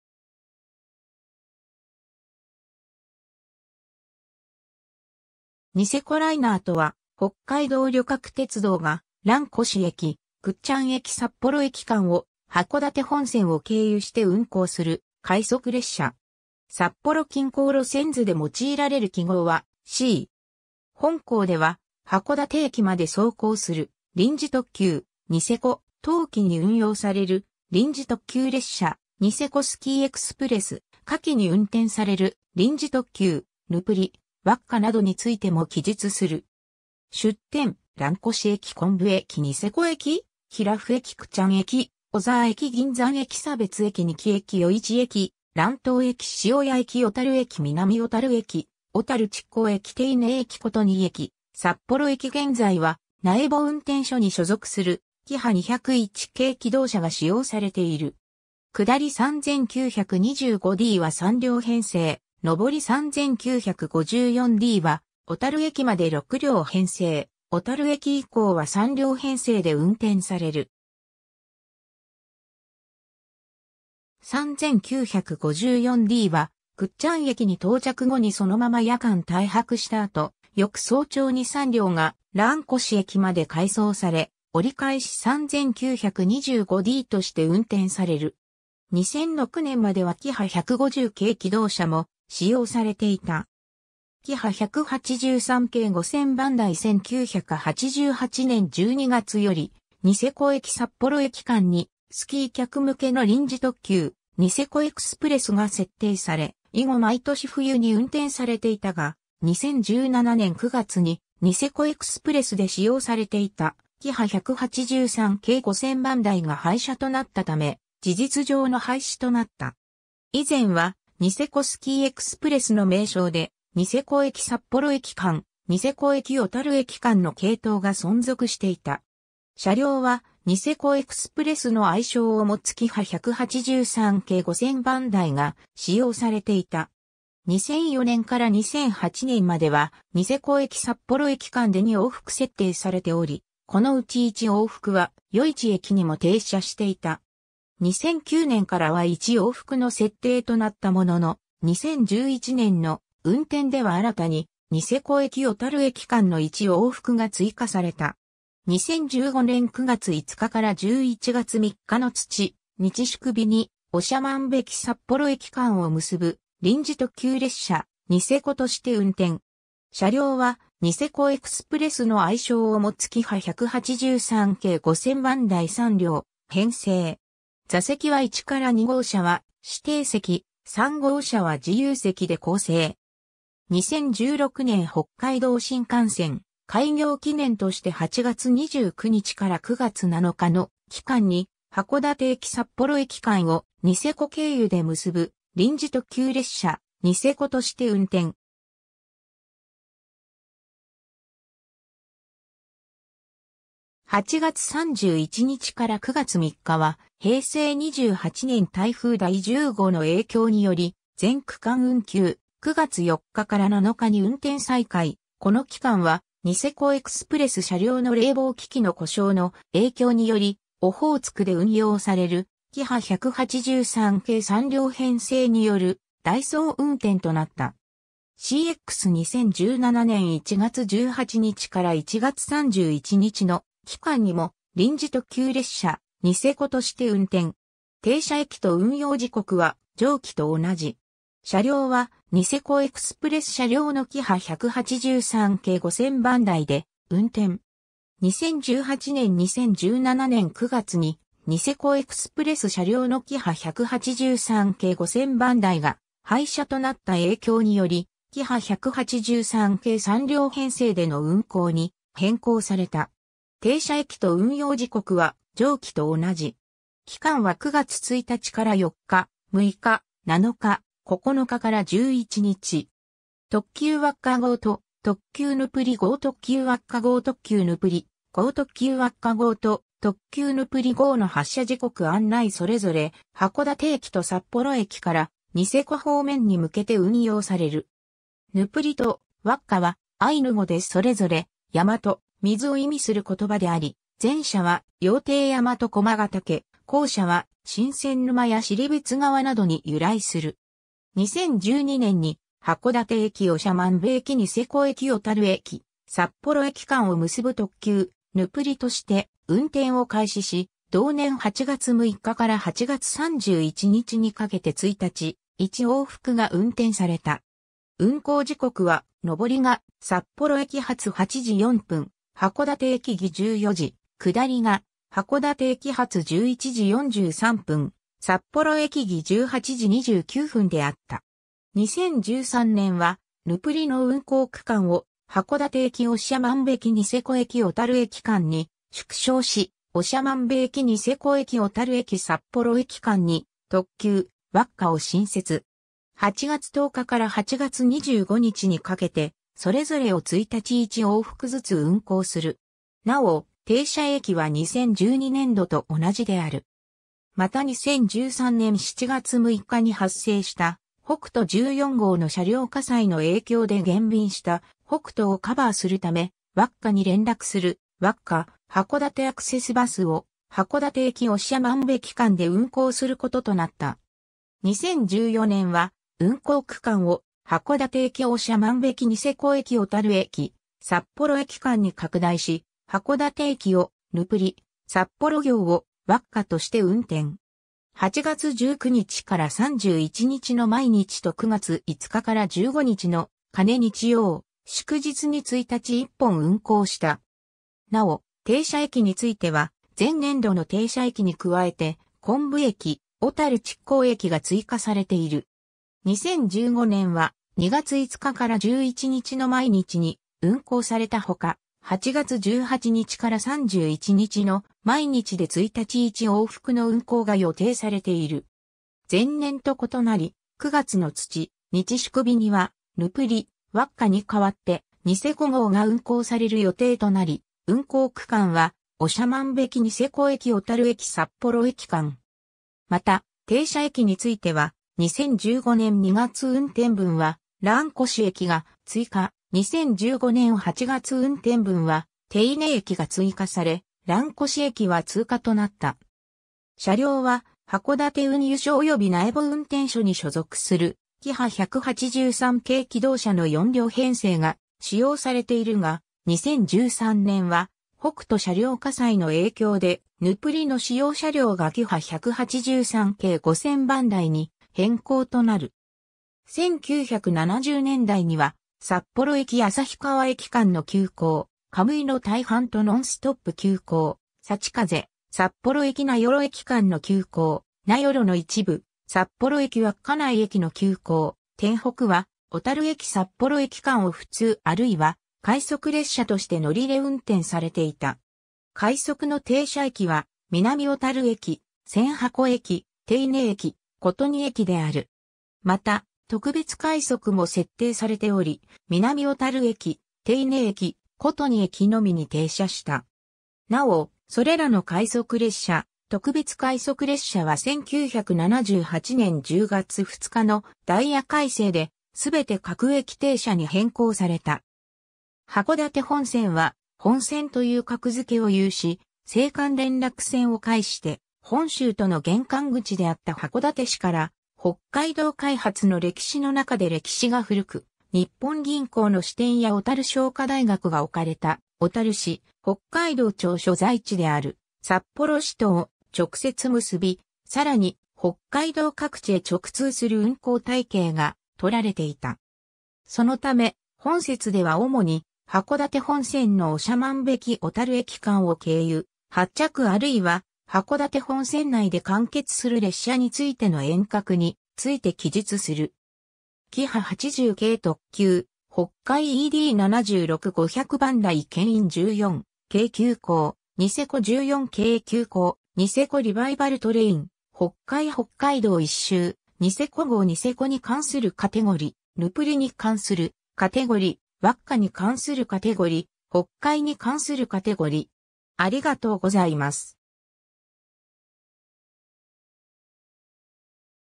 ニセコライナー
ニセコライナーとは、北海道旅客鉄道（JR北海道）が蘭越駅・倶知安駅 - 札幌駅間を函館本線を経由して運行する快速列車。
札幌近郊路線図で用いられる記号は■「C」。
 本項では、函館駅まで走行する臨時特急「ニセコ」、冬季に運用される臨時特急列車「ニセコスキーエクスプレス」、夏季に運転される臨時特急「ヌプリ」「ワッカ」などについても記述する。
 出典： 蘭越駅 → 昆布駅 → ニセコ駅 → 比羅夫駅 → 倶知安駅 - 小沢駅 - 銀山駅 - 然別駅 - 仁木駅 - 余市駅 - 蘭島駅 - 塩谷駅 - 小樽駅 - 南小樽駅 - 小樽築港駅 - 手稲駅 - 琴似駅 - 札幌駅 現在は苗穂運転所に所属するキハ201系気動車が使用されている。
下り3925Dは3両編成、上り3954Dは小樽駅まで6両編成、小樽駅以降は3両編成で運転される。
3954Dは倶知安駅に到着後にそのまま夜間滞泊した後、翌早朝に3両が蘭越駅まで回送され、折り返し3925Dとして運転される。
 2006年まではキハ150形気動車も使用されていた。
 キハ183系5000番台（函館本線稲積公園駅、2010年12月25日） 1988年12月より、ニセコ駅 - 札幌駅間（一部は新千歳空港駅まで運転）に、スキー客向けの臨時特急「ニセコエクスプレス」が設定され、以後毎年冬に運転されていたが、2017年9月に「ニセコエクスプレス」で使用されていたキハ183系5000番台が廃車となったため、事実上の廃止となった。
以前は「ニセコスキーエクスプレス」の名称で、ニセコ駅 - 札幌駅間・ニセコ駅 - 小樽駅間の系統が存続していた。
 車両は「ニセコエクスプレス」の愛称を持つキハ183系5000番台が使用されていた。
2004年 - 2008年まではニセコ駅 - 札幌駅間で2往復設定されており、このうち1往復は余市駅にも停車していた。
2009年からは1往復の設定となったものの、2011年の運転では新たにニセコ駅 - 小樽駅間の1往復が追加された。
 2015年（平成27年）9月5日から11月3日の土・日・祝日に長万部駅 - 札幌駅間を結ぶ臨時特急列車「ニセコ」として運転。
 車両は「ニセコエクスプレス」の愛称を持つキハ183系5000番台3両編成。
座席は1 - 2号車は指定席、3号車は自由席で構成。
 2016年（平成28年）北海道新幹線開業記念として8月29日から9月7日の期間に函館駅 - 札幌駅間をニセコ経由で結ぶ臨時特急列車「ニセコ」として運転。
 8月31日から9月3日は平成28年台風第10号の影響により、全区間運休、9月4日から7日に運転再開、この期間は「ニセコエクスプレス」車両の冷房機器の故障の影響により、オホーツクで運用されるキハ183系3両編成による代走運転となった。
 Cｘ 2017年（平成29年）1月18日から1月31日の期間にも臨時特急列車「ニセコ」として運転。
停車駅と運用時刻は上記（2016年）と同じ。
車両は「ニセコエクスプレス」車両のキハ183系5000番台で運転。
 2018年（平成30年）2017年9月に「ニセコエクスプレス」車両のキハ183系5000番台が廃車となった影響により、キハ183系3両編成での運行に変更された。
期間は9月1日(土)～4日(火)、6日(木)、7日(金)、9日(日)～11日(火)。
 特急ワッカ号と特急ヌプリ号（函館本線倶知安駅、2013年8月13日） 特急ワッカ号（函館本線倶知安駅、2013年8月13日） 特急ヌプリ号（函館本線倶知安駅、2013年8月13日） 特急ワッカ号と特急ヌプリ号の発車時刻案内（函館本線倶知安駅、2013年8月13日） それぞれ函館駅と札幌駅から、ニセコ方面に向けて運用される。
 「ヌプリ」と「ワッカ」はアイヌ語でそれぞれ「山」と「水」を意味する言葉であり、前者は羊蹄山と駒ヶ岳、後者は神仙沼や尻別川などに由来する。
 2012年（平成24年）に函館駅 - 長万部駅 - ニセコ駅 - 小樽駅 - 札幌駅間を結ぶ特急「ヌプリ」として運転を開始し、同年8月6日 - 8月31日にかけて1日1往復が運転された。
運行時刻は上りが札幌駅発8:04・函館駅着14:00、下りが函館駅発11:43・札幌駅着18:29であった。
 2013年（平成25年）は「ヌプリ」の運行区間を函館駅 - 長万部駅 - ニセコ駅 - 小樽駅間に縮小し、長万部駅 - ニセコ駅 - 小樽...